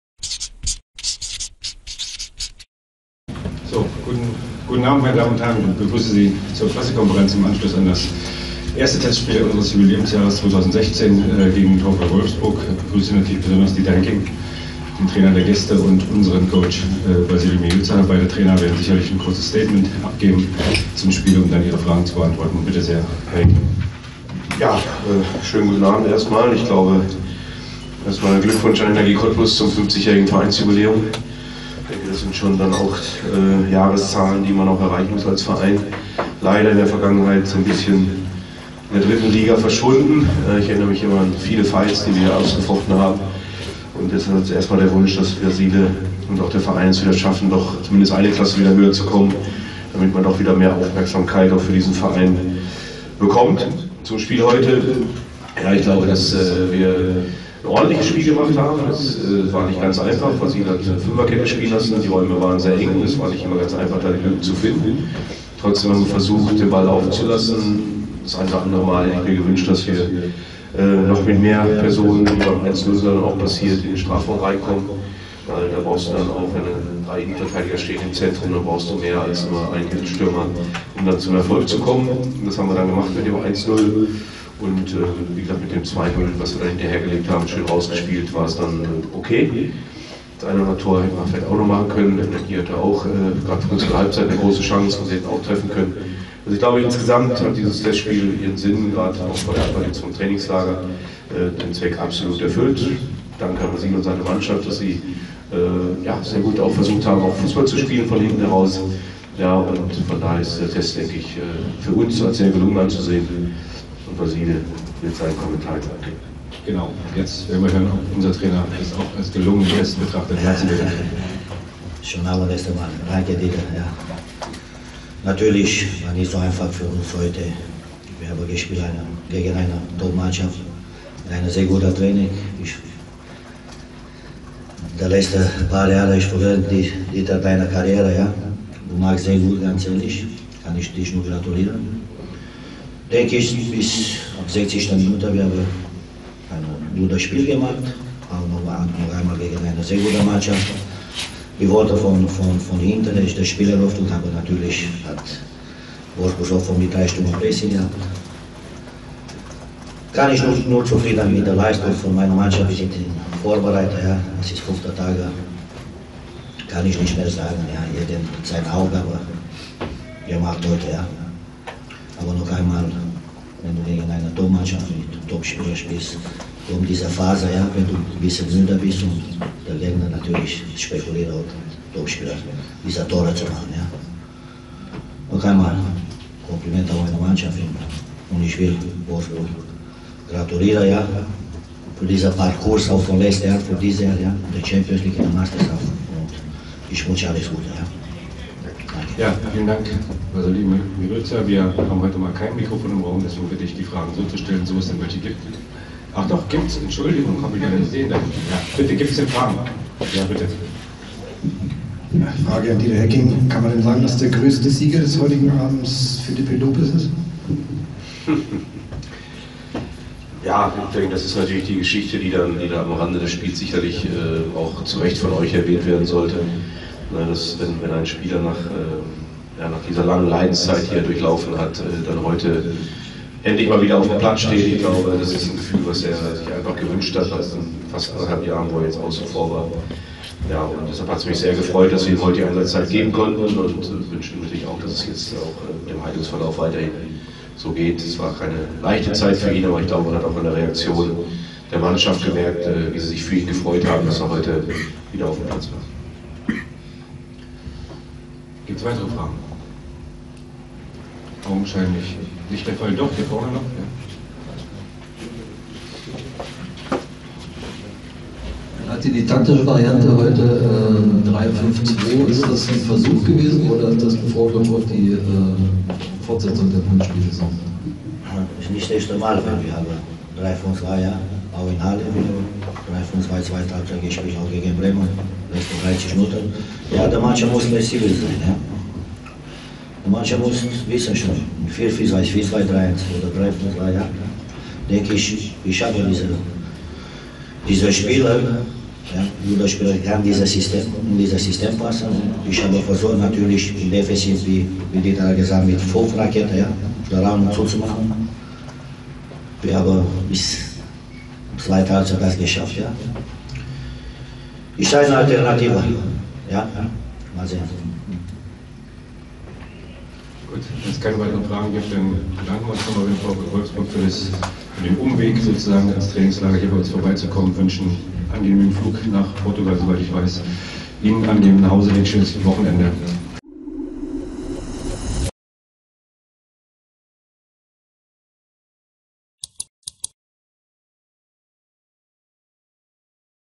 PK nach dem Testspiel zwischen Energie Cottbus und VfL Wolfsburg
Viel Lob von beiden Seiten. Die Trainer sind zufrieden. Vasile Miriuta und Dieter Hecking finden nach dem 0:2 zwischen Energie Cottbus und VfL Wolfsburg positive Worte zu ihren Teams und dem Gegner.